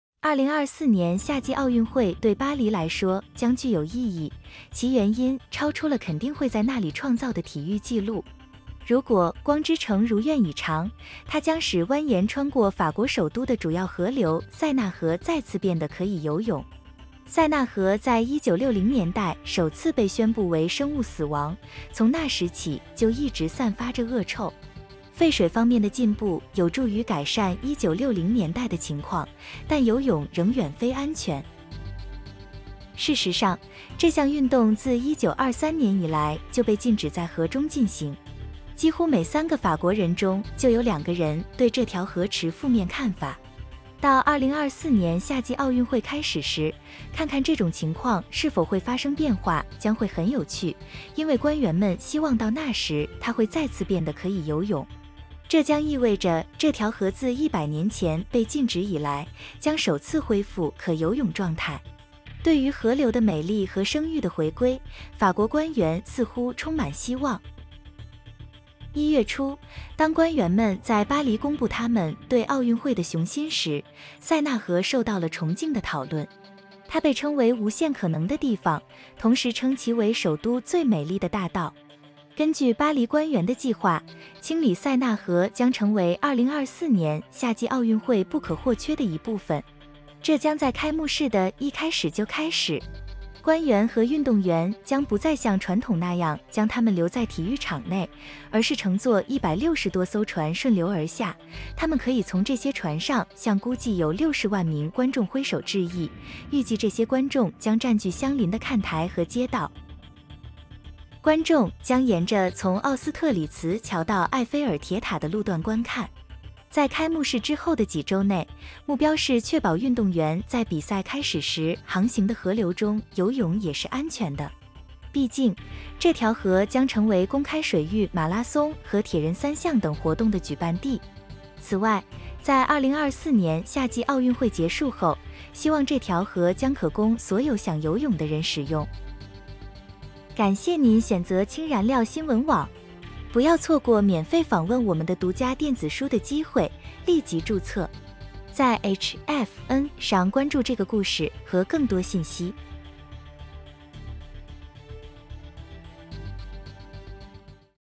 [Chinese] Paris aims to clean up the Seine in time for the 2024 Summer Olympic Games
Officials in France have set an ambitious goal of cleaning up the river to make it swimmable...